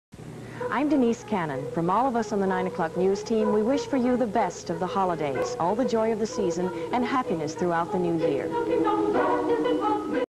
WGN-TV: Season Greetings ID (December 1984)
WGN Season Greetings Identification featuring Denise Cannon on Chicago's Very Own.

IN THE WGN-TV ARCHIVES!